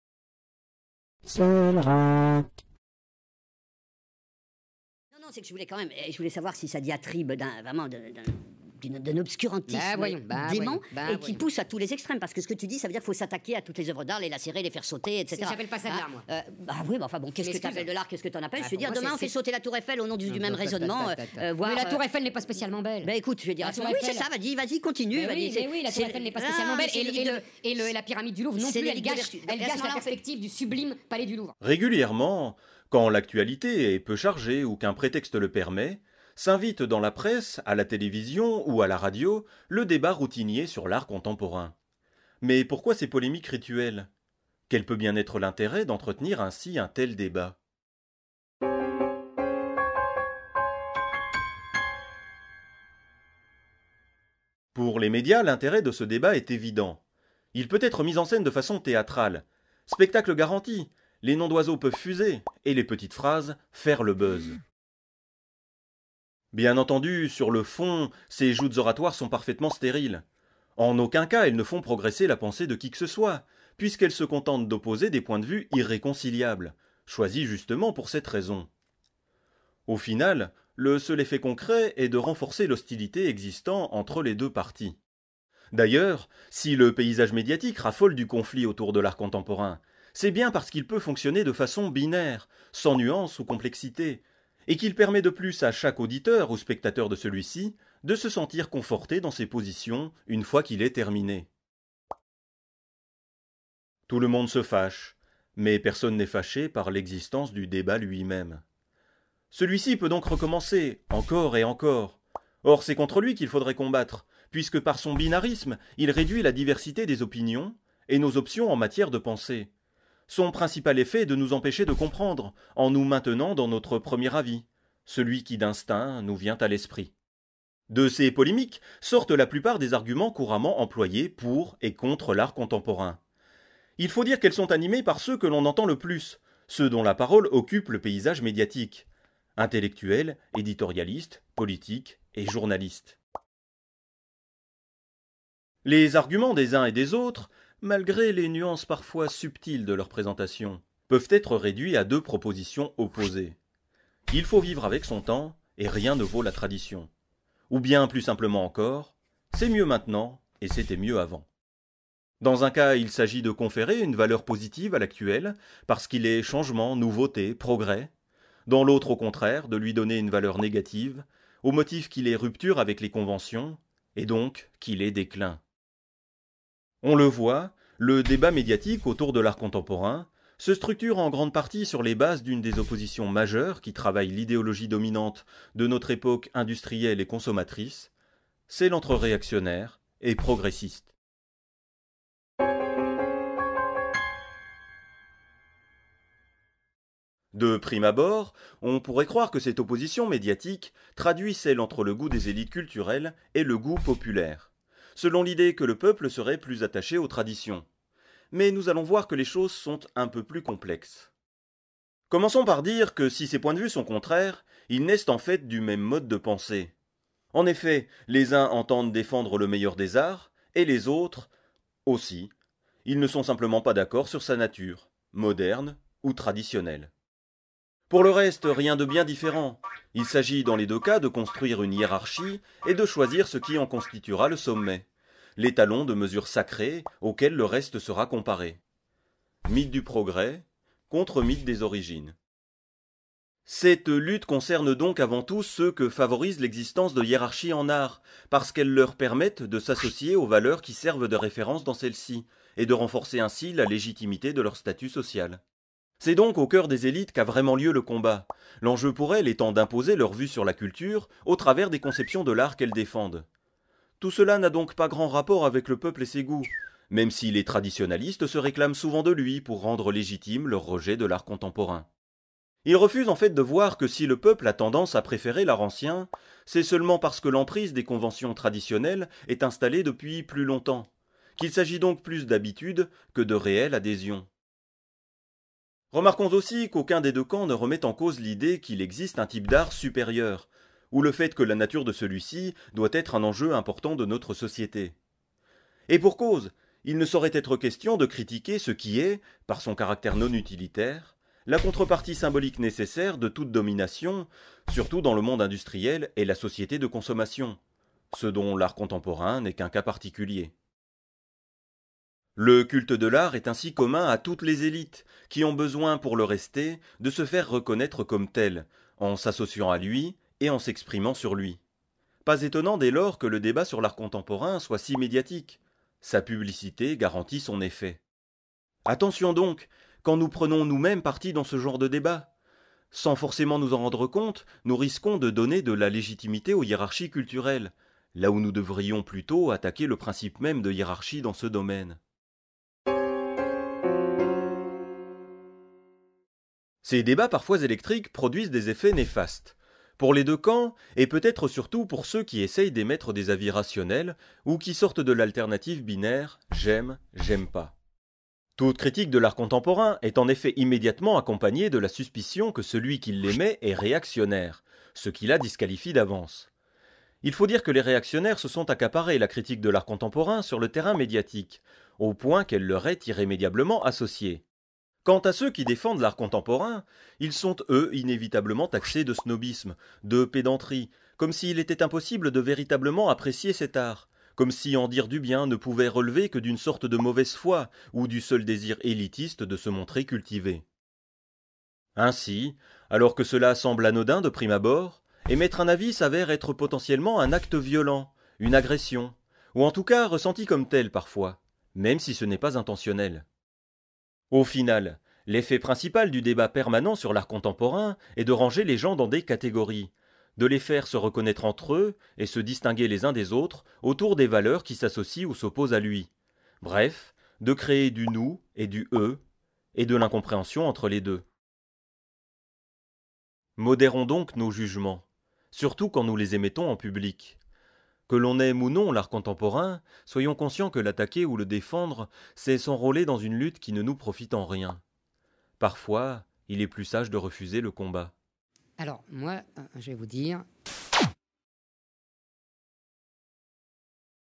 34-"Art contemporain". Huitième partie : un débat médiatique.
Les débats sur l’art contemporain dans les médias sont sources d’invectives et de colère. Pour quelles raisons ? Quelques éléments de réponse dans cette vidéo.

Lorsqu'elles ne sont pas produites par moi-même, les images proviennent de wikimedia commons.